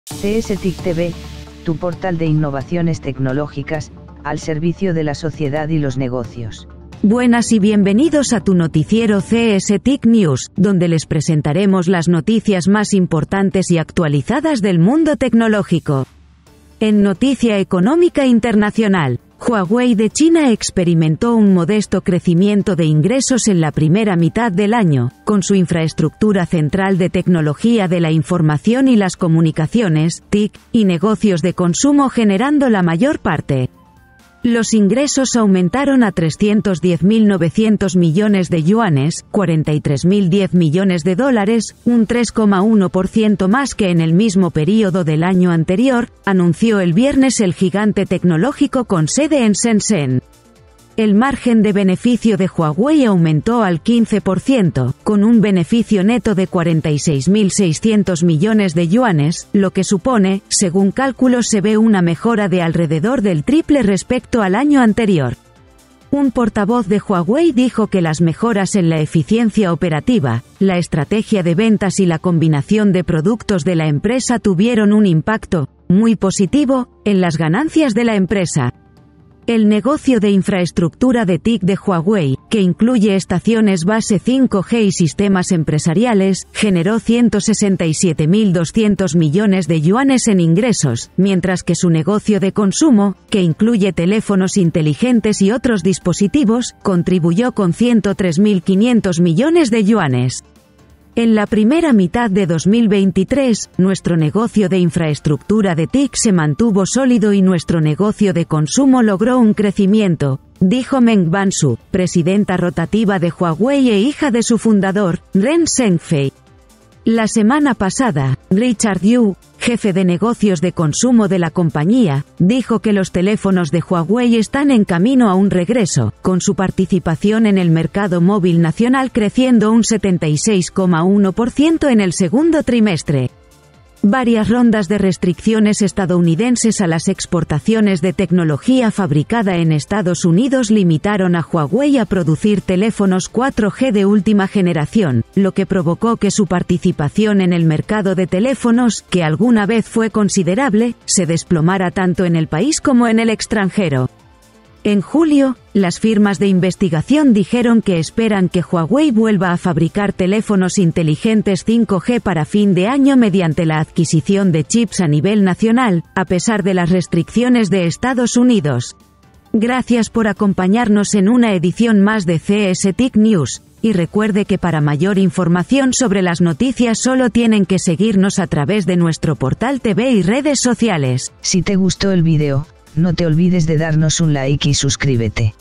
Huawei informa un pequeño aumento de ingresos en la parte posterior de los negocios principales
Huawei Technologies de China experimentó un modesto crecimiento de ingresos en la primera mitad del año, con su infraestructura central de tecnología de la información y las comunicaciones (TIC) y negocios de consumo generando la mayor parte. Los ingresos aumentaron a 310.900 millones de yuanes (43.010 millones de dólares), un 3,1% más que en el mismo período del año anterior, anunció el viernes el gigante tecnológico con sede en Shenzhen. Ver las imágenes en nuestro Portal Tv.
Fuente: reuters